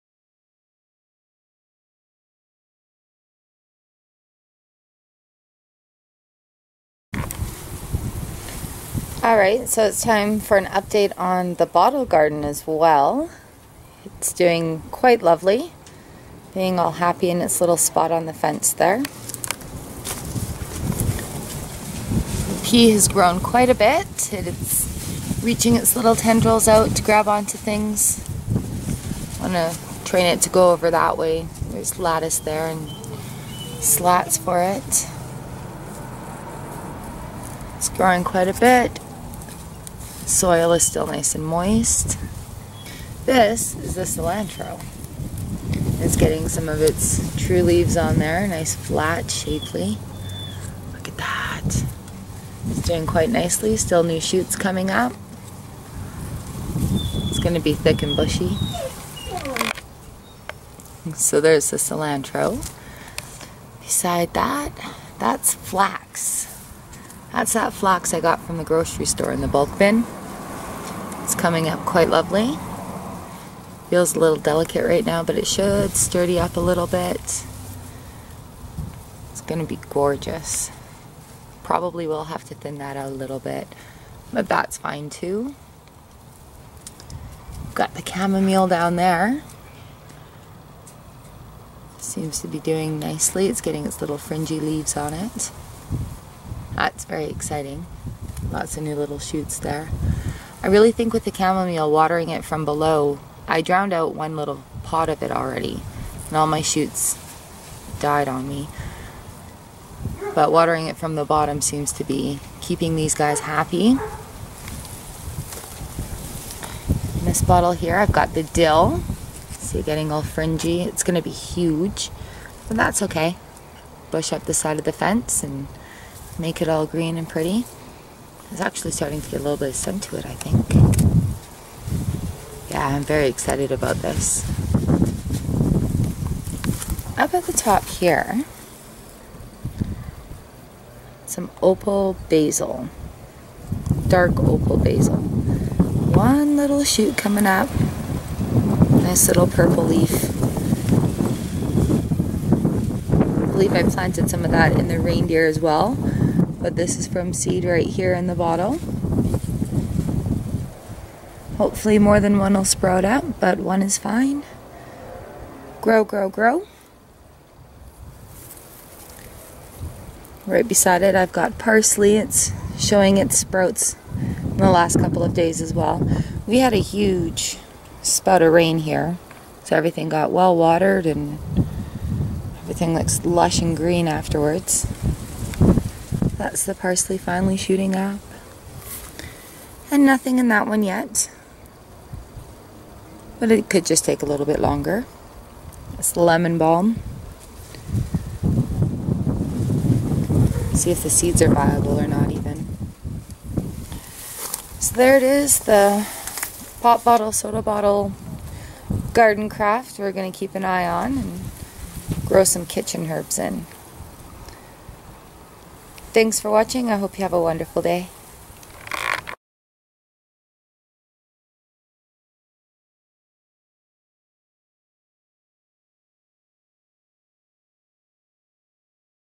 Soda Pop Bottle Garden Update May 25 2013
The soda pop bottle garden is showing some wonderful growth and I wanted to share it with you!! Have a look at the pea, cilantro, flax, chamomile, dill, dark opal basil and parsley. This is just a taste of how wonderful it will look in full growth!
Chox =)